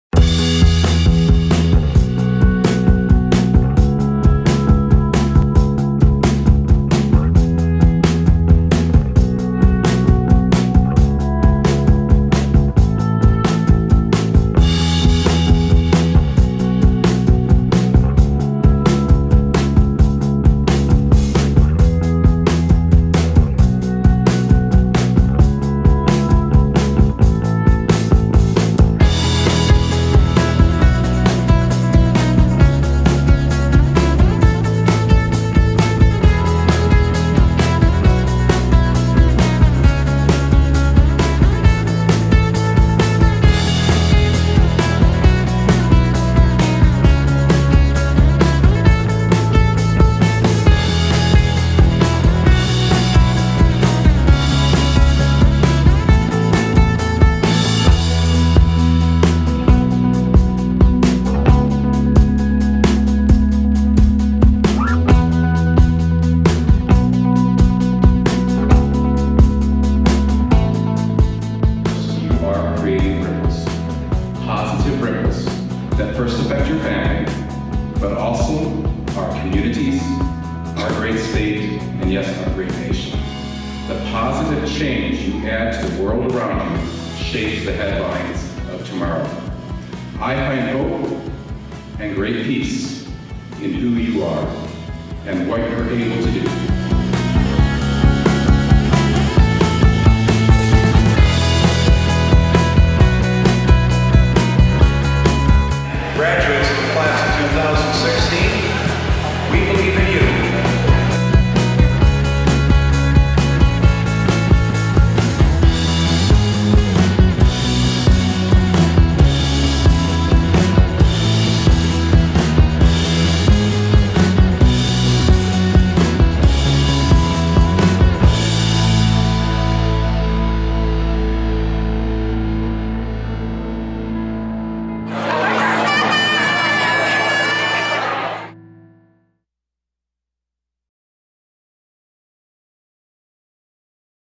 Minnesota West Commencement 2016
This is a compilation of photos and video footage from all five of the Minnesota West Community & Technical College Commencement Ceremonies in 2016. Congratulations graduates and thank you for choosing Minnesota West!